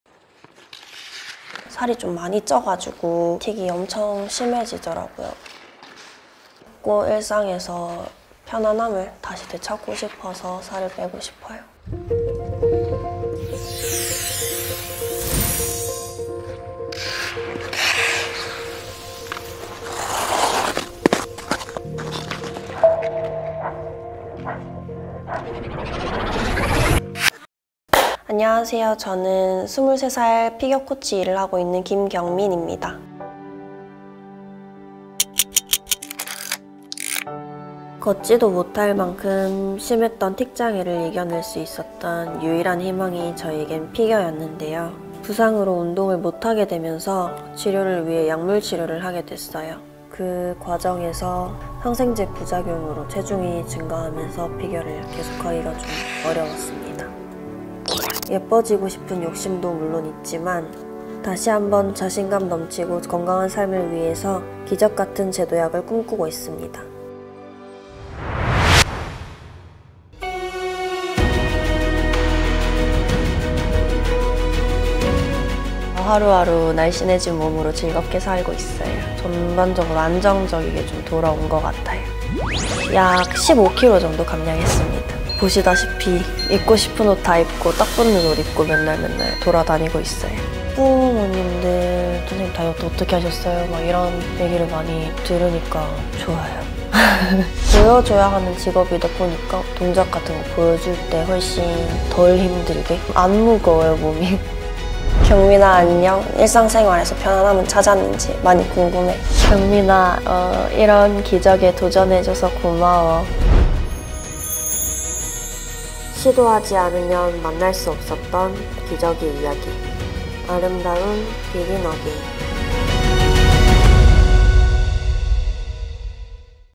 대구365mc를 픽한 한 피겨선수, 어떻게 됐을까👀
이후 그녀는 다시 피겨선수로 복귀하였는데요.
대구365mc는 늘 그녀의 열정과 끈기를 응원합니다~:)